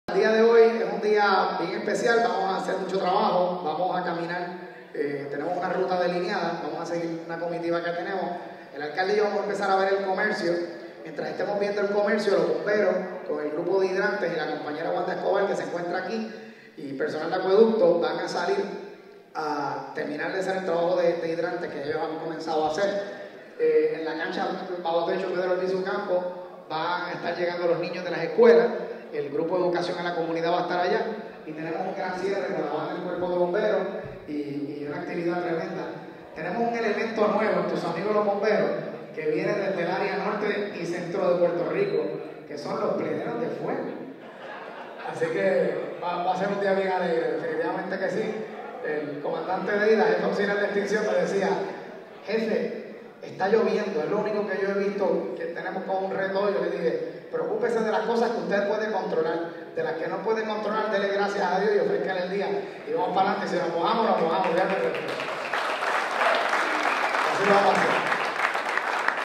Tus Amigos Los Bomberos, Yabucoa
El Jefe del Cuerpo de Bomberos Ángel A. Crespo Ortiz en unión al Alcalde de Yabucoa Hon. Rafael "Rafy" Surillo celebraron el evento de impacto "Tus Amigos Los Bomberos" se inspeccionaron los 232 hidrantes del Pueblo de Yabucoa quedando el 94% de estos en condiciones operables. Se educaron estudiantes de las Escuelas Elementales con Show Educativo, se instalaron 100 Detectores de Humo en Hogares de Envejecientes, Se inspeccionaron los comercios, se Juramentó el 5to Club de Bomberitos Escolares en la Escuela S.U. Cristobal del Campo del Bo. Limones y se devalaron las letras con el nombre del Sgto. Teófilo Quiñones Claudio "Cachiro" nombrando así la Estación de Bomberos de Yabucoa Oficialmente. Hubo un hermosa actividad de confraternización con los Bomberos Retirados. En ƒin, todo un éxito.